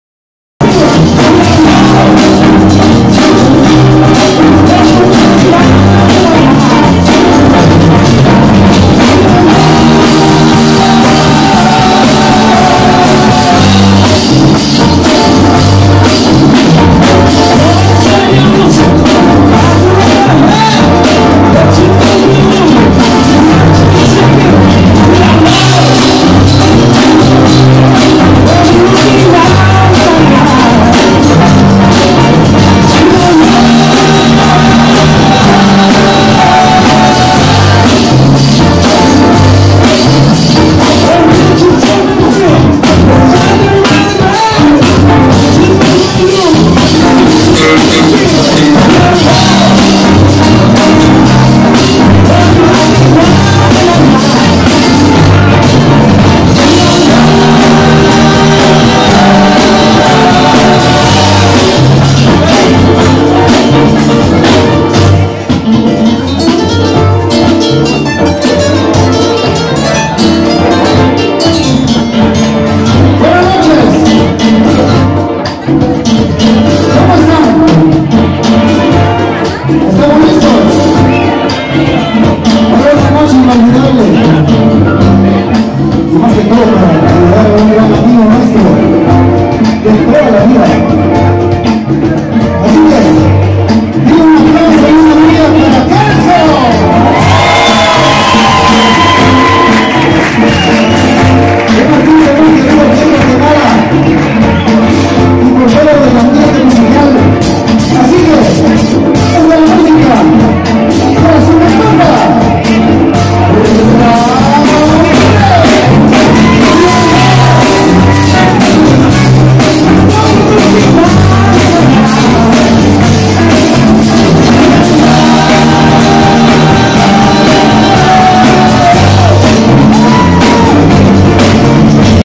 Super concierto UNIDOS POR KELSON con la SUPERBANDA RAUDALES.
Una exploción de magia y recuerdos a cargo de los mejores músicos nacionales.Gracias a todos los participantes por su apoyo.